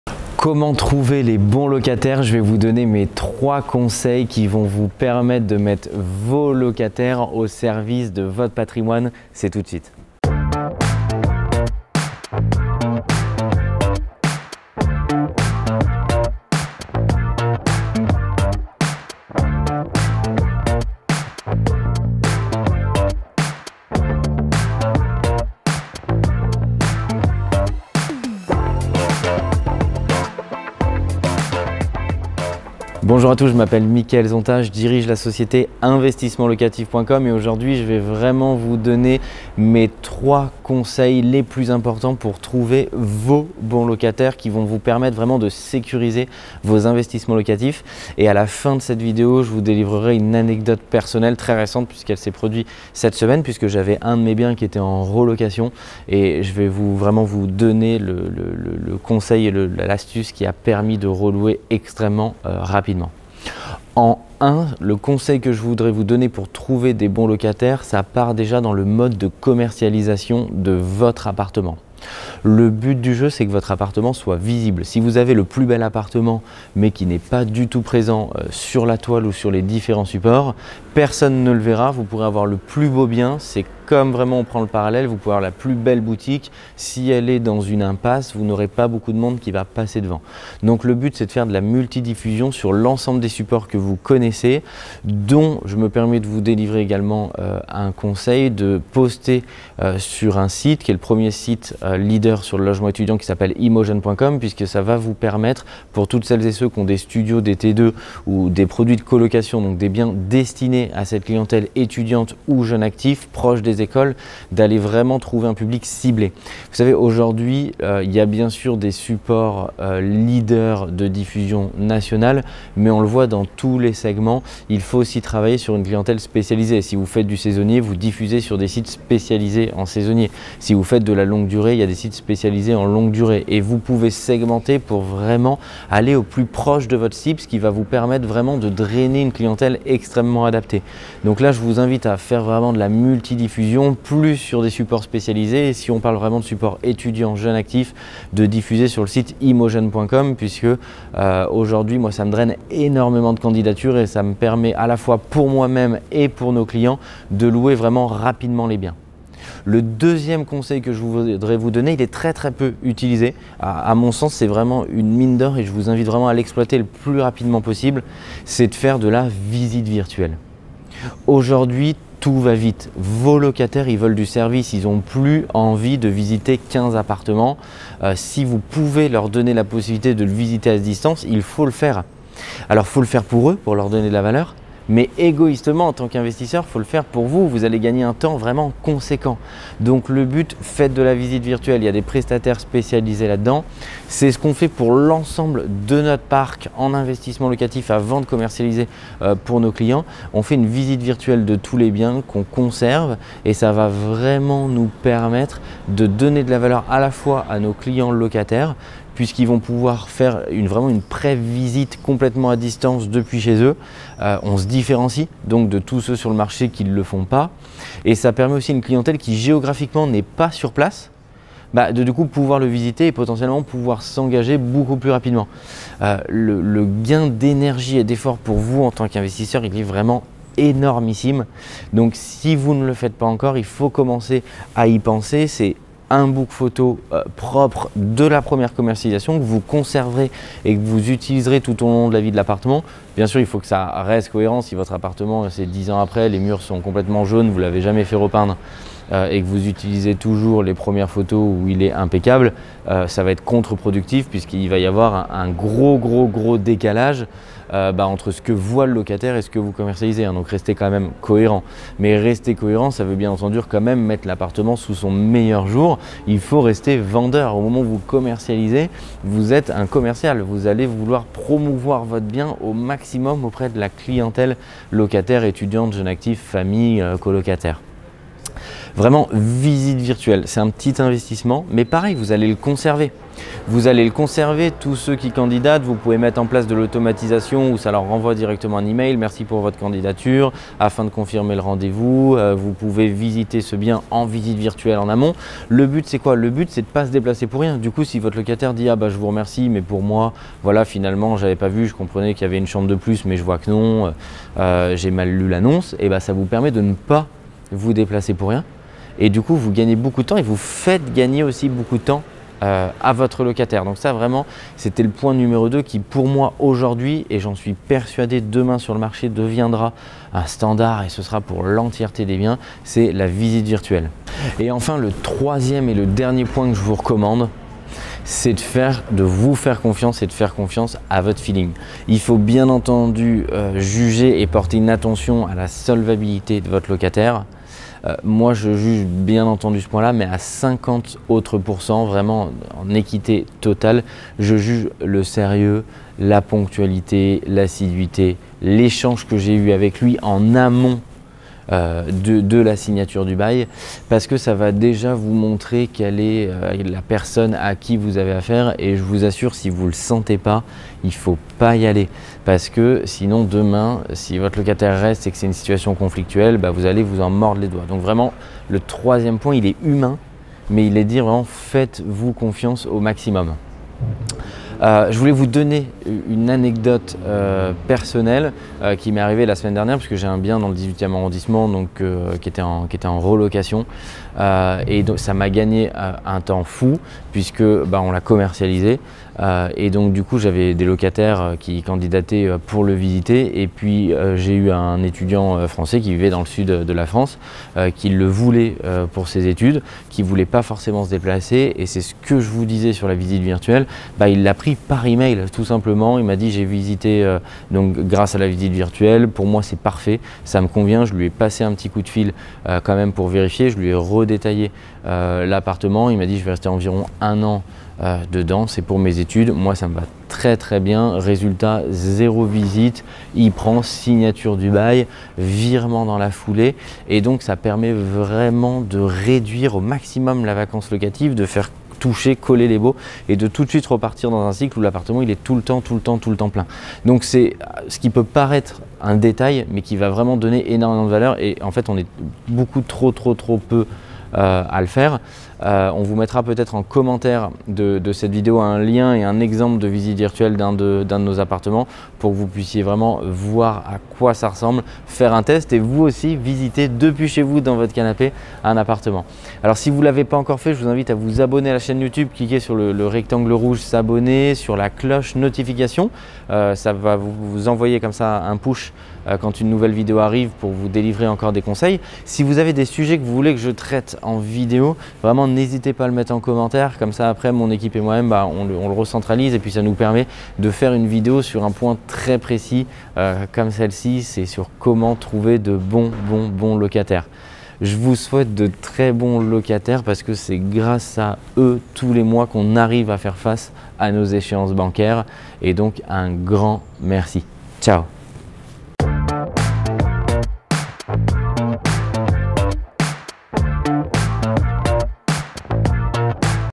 Gestion locative : Trouver un BON locataire rapidement ! ✔️💲
► ✅ Créez VOTRE empire immobilier grâce à mes conseils : Vidéos + Guide Gratuit

Les locataires sont la pierre angulaire de l’investissement locatif. Ils permettent de sécuriser vos investissements locatifs. Mais trouver des bon locataires nécessite un investissement de votre part.

Vous venez d’investir dans un logement locatif et vous cherchez un occupant sérieux pour sécuriser votre projet immobilier  ou peut-être que vous êtes bailleur depuis longtemps et vous en avez assez de tomber sur des locataires négligents.

Découvrez mes méthodes pour faciliter vos recherches. Et ainsi mettre vos locataires au service de votre patrimoine immobilier.

A bientôt.

Bon visionnage !

★ MES PAGES FACEBOOK :
Suivez toute mon actualité sur la page facebook :